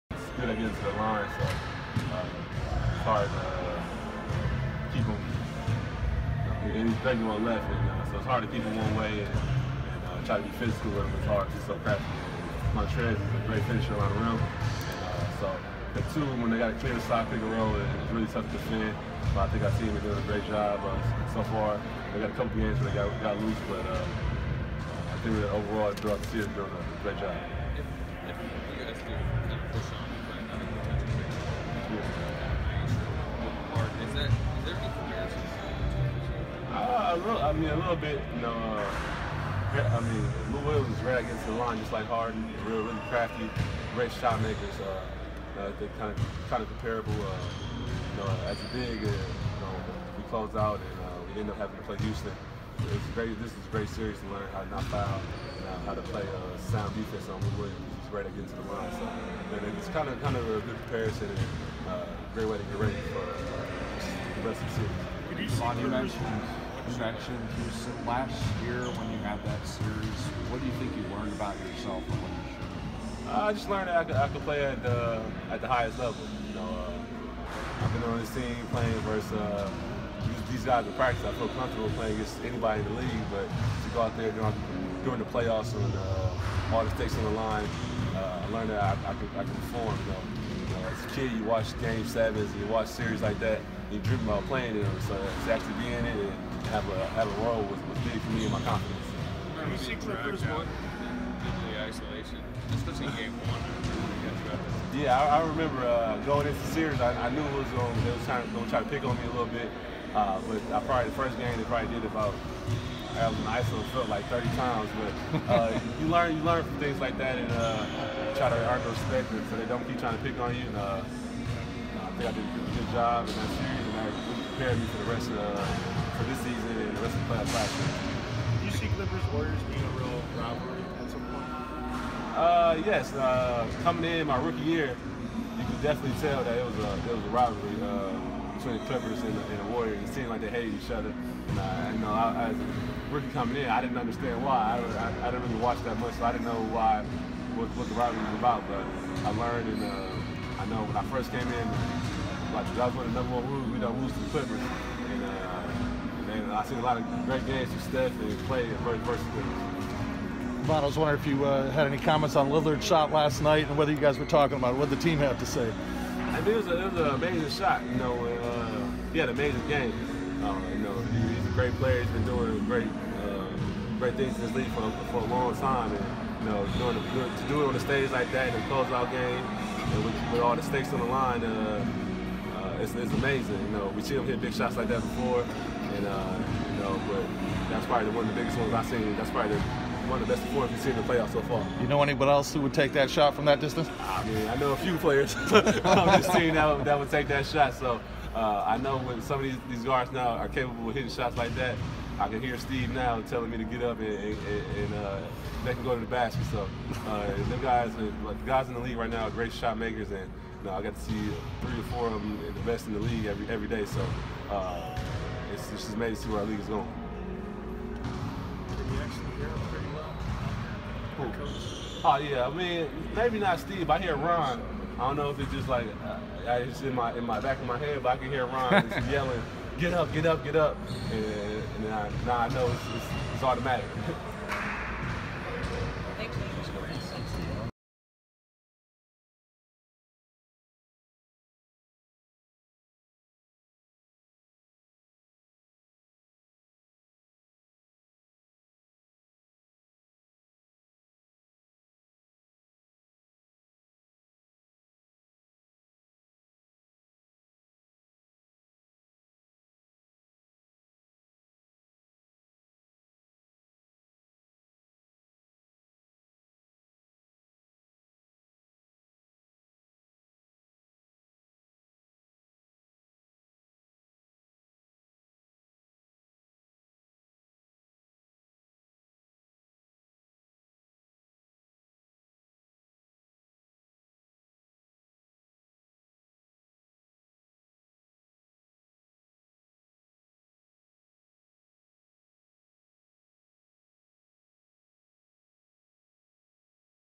Kevin Looney On How Clippers Series Will Help Destroy Rockets
Prior to Game 5 vs. Clippers, Warriors' Kevon Looney explains the real benefit of the scrappy Clippers team and how they will impact the rest of the 2019 NBA Playoffs for the Warriors.